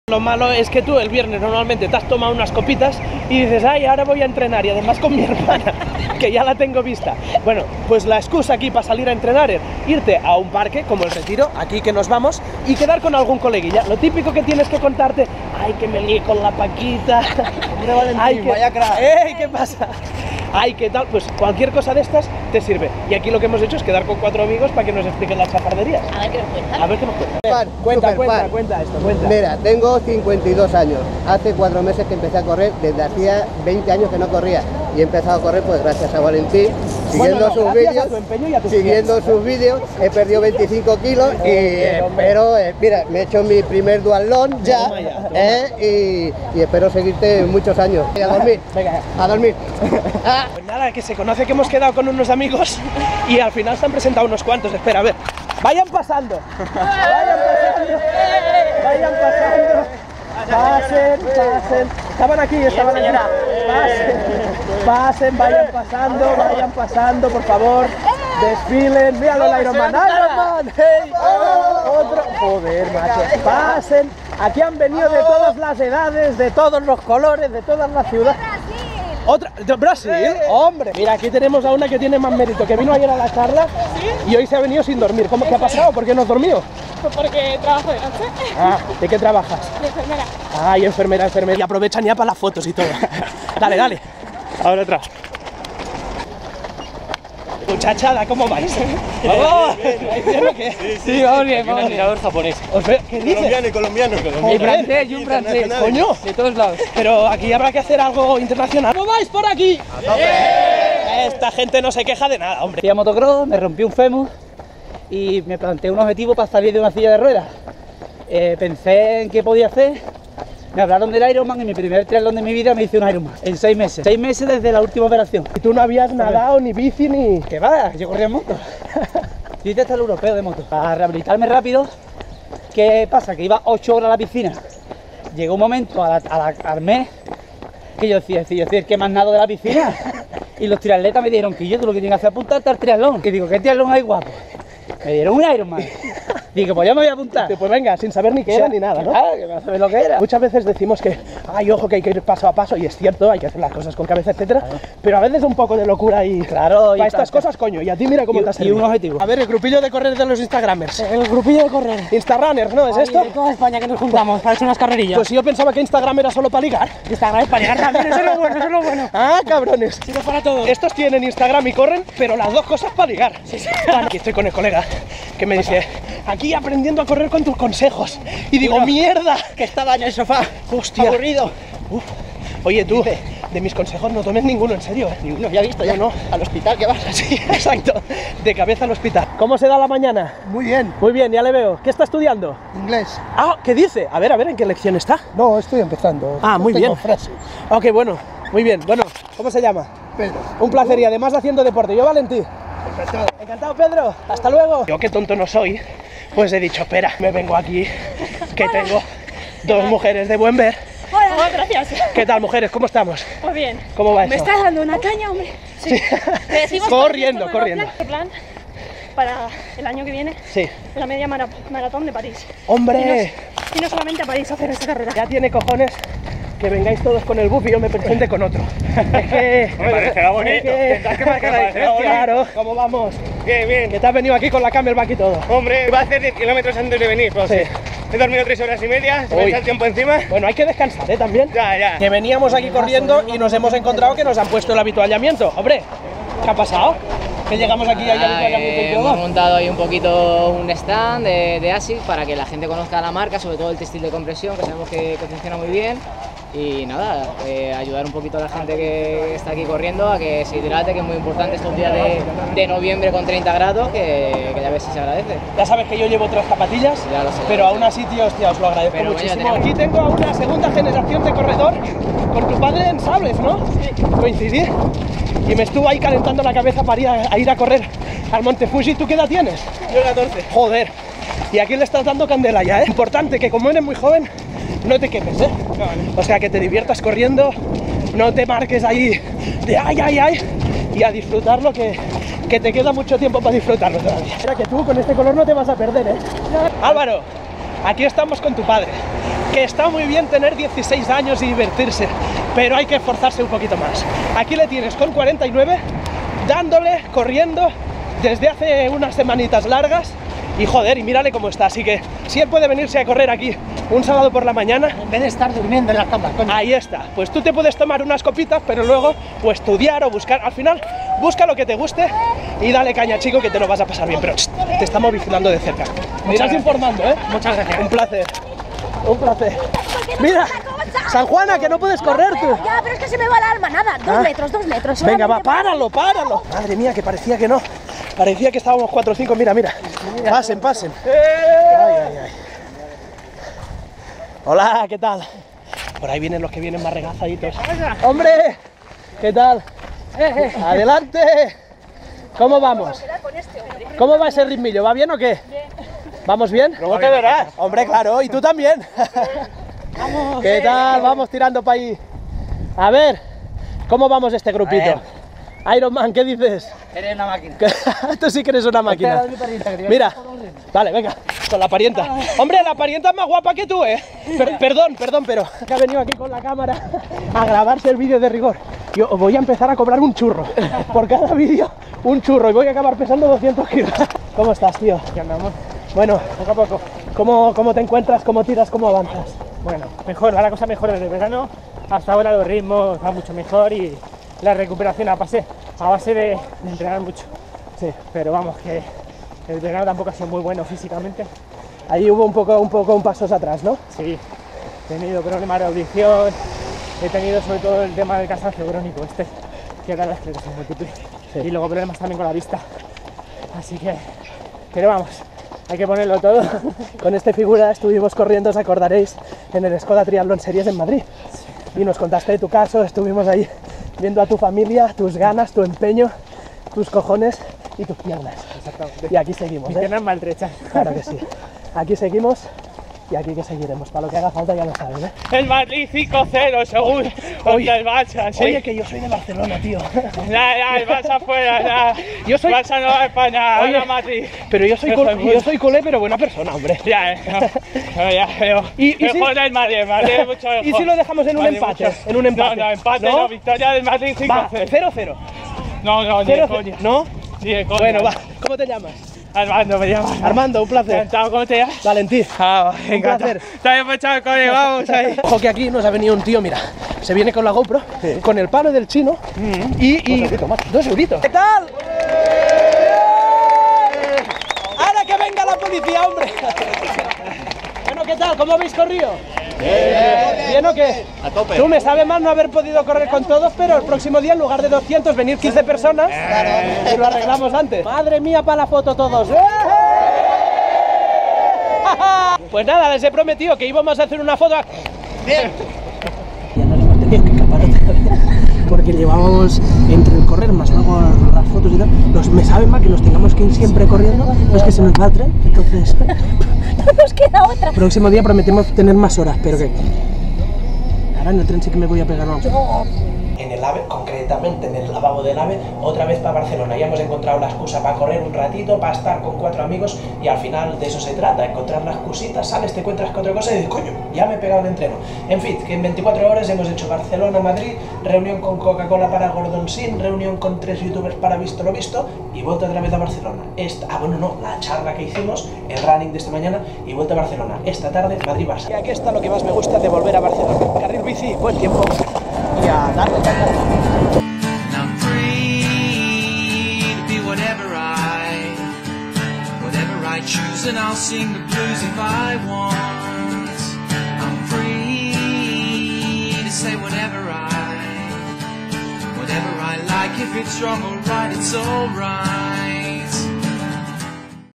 Truco Para Correr Sábado: Quedear Con Colegas
Quedada en Madrid con algunos cientos de personas :) Y truco para correr un sábado por la mañana!